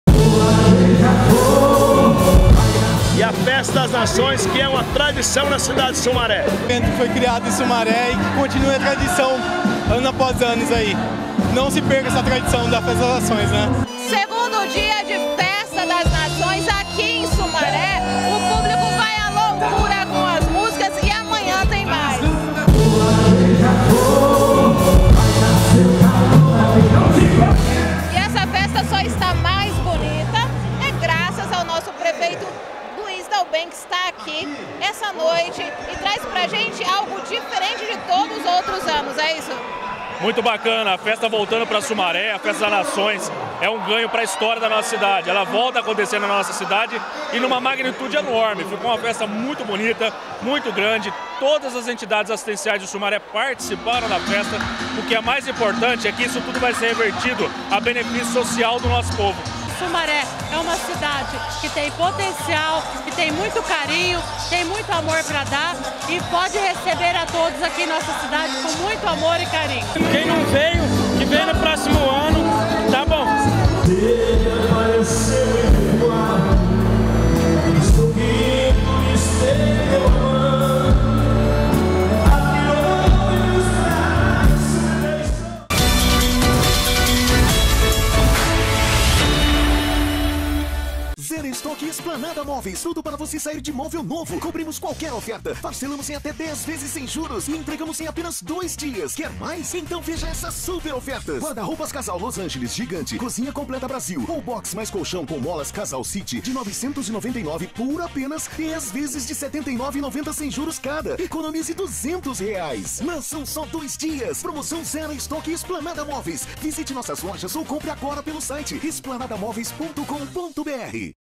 FESTA DAS NAÇÕES EM SUMARÉ
A volta da Festa das Nações foi um sucesso nesse fim de semana em Sumaré-SP. Veja a cobertura completa da TVHORTOLÂNDIA nesse vídeo.